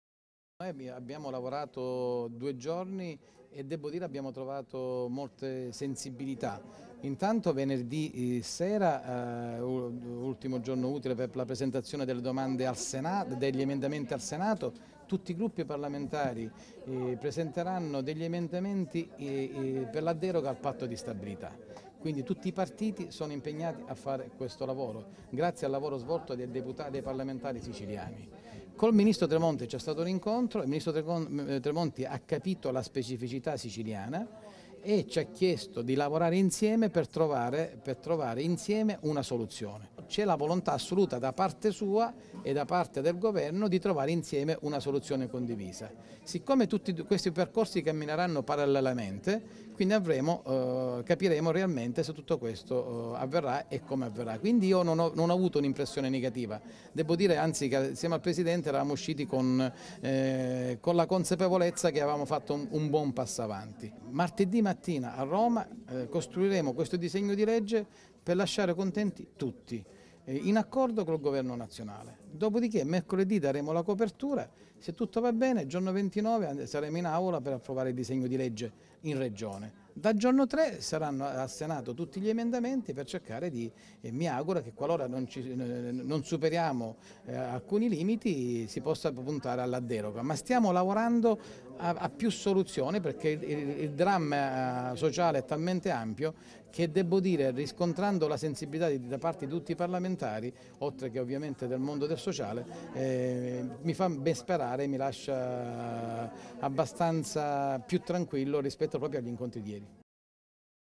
SICILIA TV (Favara) Precari. Intervista dell'assessore Leanza
Il ministro Tremonti ha fornito al governo siciliano le indicazioni utili ad avviare un processo di stabilizzazione dei precari che possa superare l'esame tecnico e politico del parlamento nazionale.